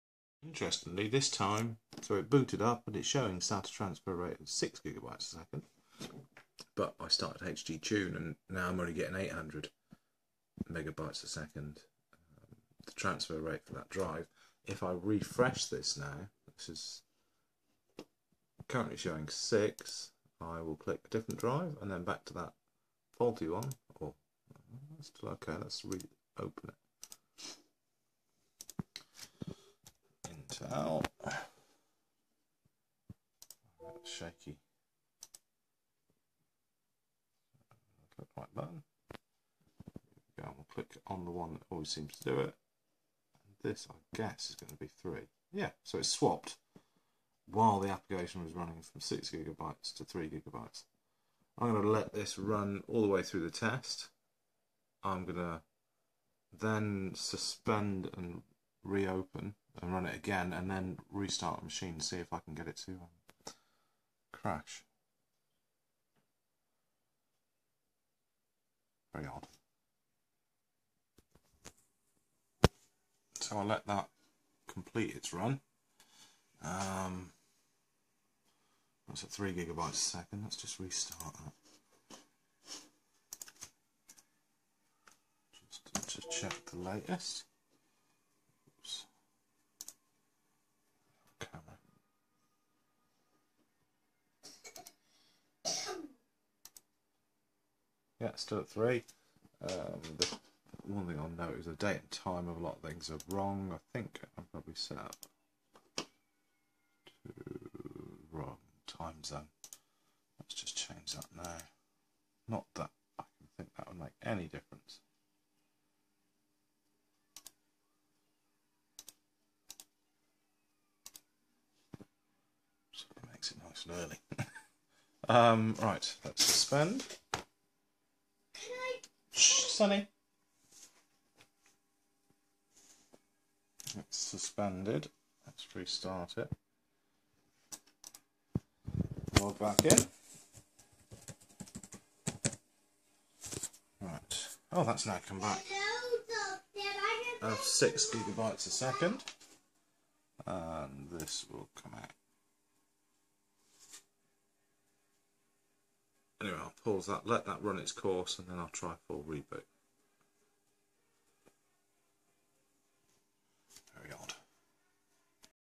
MSI GT70 2OD - Super Raid 2 - running slow
MSI GT70 2OD - Super Raid 2.

Initially its running at 800MB/s due to one of the drives appears as SATA 3GB/s

At the end of the video it runs at 1600MB/s as the drive has swapped to SATA 6GB/s